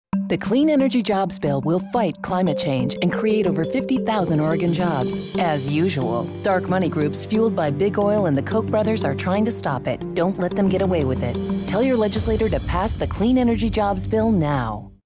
Tell Lawmakers to hold polluters accountable: Pass HB 2020
The Clean Energy Jobs bill holds big polluters accountable and helps fight climate change. That's why Big Oil and Koch Brothers-backed dark money groups are trying to stop it. Help us fight back